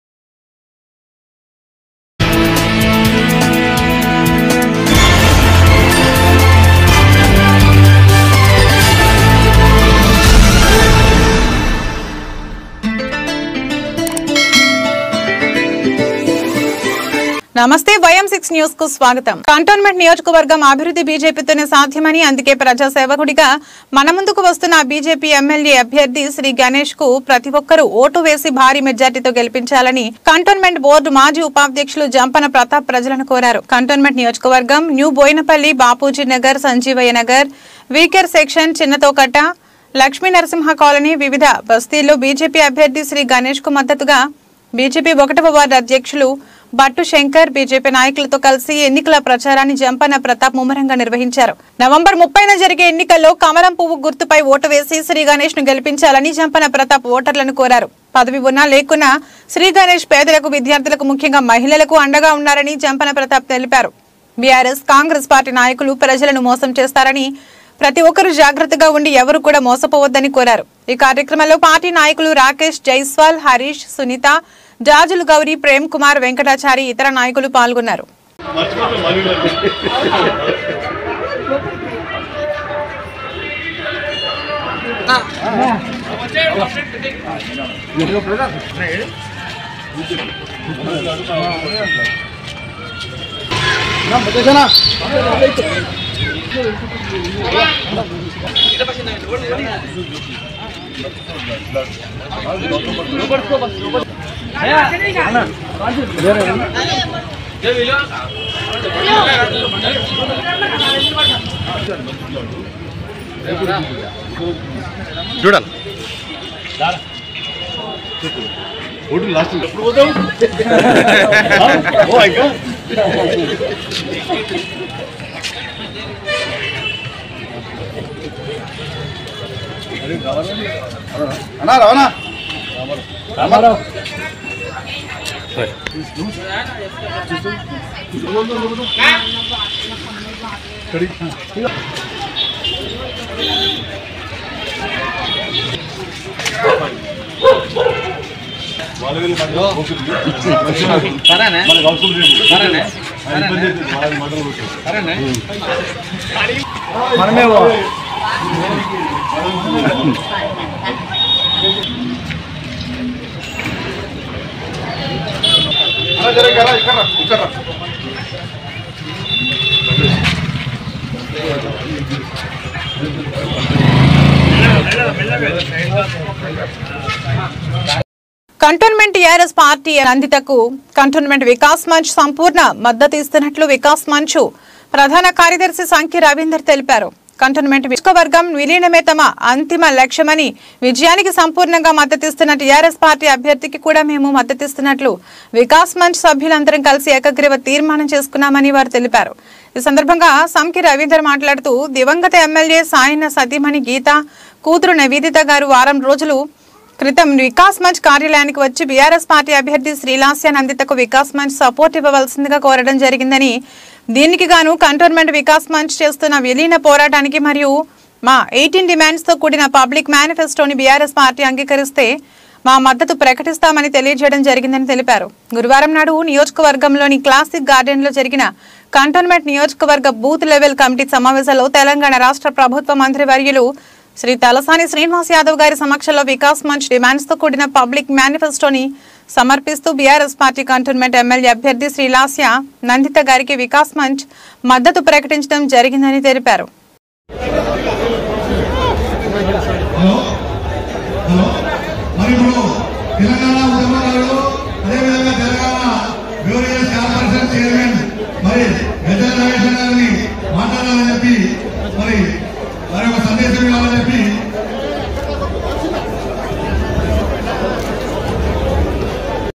జోరుగా కంటోన్మెంట్ ఎన్నికల ప్రచారం. YM6_News_Bulletin...
లాస్య_నదితకు కంటోన్మెంట్ వికాస్ మంచ్ సంపూర్ణ మద్దతు.

YM6_ News_ Bulletin.